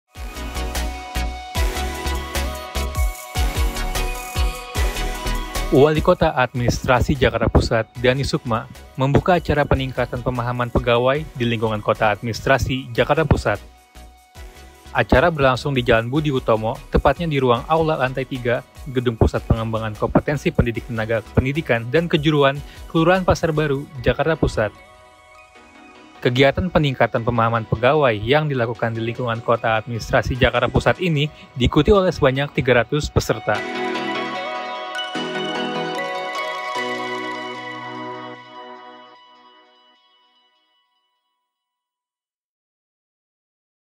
Buka Peningkatan Pemahaman Pegawai, Wali Kota: Setiap Kebijakan Baru Harus Segera Disosialisasikan
Wali Kota Administrasi Jakarta Pusat Dhany Sukma membuka acara Peningkatan Pemahaman Pegawai di ingkungan Kota Administrasi Jakarta Pusat yang berlangsung di Ruang Aula Lantai III Gedung Pusat Pengembangan Kompetensi Pendidik Tenaga Kependidikan dan Kejuruan (P2KPTK2) Jakarta Pusat, Kelurahan Pasar Baru, Kamis (1/12) lalu.

Peningkatan pemahaman pegawai kali ini mensosialisasikan Perka BKN Nomor 7 Tahun 2021 dan Perka BKN Nomor 7 Tahun 2022 tentang cuti PNS dan PPPK serta Peraturan Gubernur (Pergub) Nomor 98 Tahun 2021 tentang kode etik dan kode perilaku Aparatur Sipil Negara (ASN) di ingkungan Pemerintah Provinsi DKI Jakarta.

Kegiatan kali ini juga diadakan untuk mengupdate peraturan perundang-undangan yang begitu cepat perubahannya seperti sebelumnya Perka BKN Nomor 24 Tahun 2017 yang berubah menjadi Perka BKN Nomor 7 Tahun 2021.

Kegiatan ini diikuti 300 peserta yang terdiri dari kepala sekolah, kepala tata usaha, maupun kepala seksi kecamatan yang ada di wilayah Kota Administrasi Jakarta Pusat dengan beberapa materi yang disampaikan oleh narasumber dari BKN dan Badan Kepegawaian Provinsi DKI Jakarta.

Tonton juga JPinfo lainnya agar kalian mengetahui informasi seputar Jakarta Pusat di playlist Info Jakarta Pusat atau klik